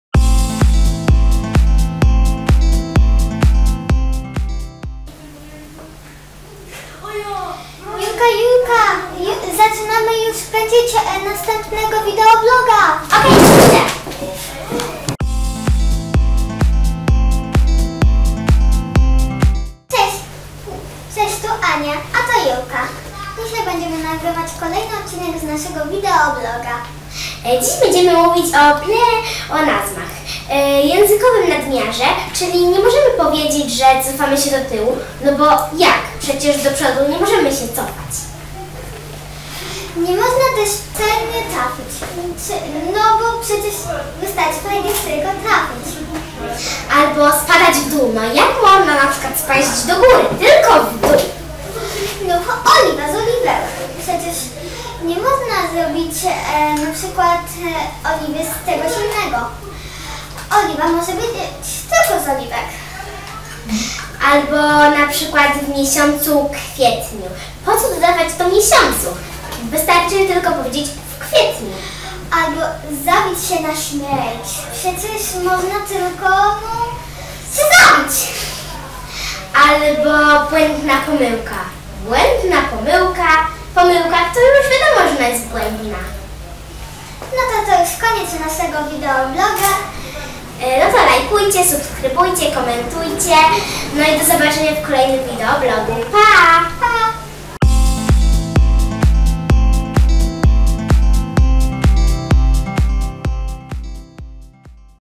masło maślane
Julka z Anią opowiadają o tym, czym jest pleonazm i czy można cofać sie do tyłu. Film powstał w ramach projektu Bądż stylowy-odlotowy.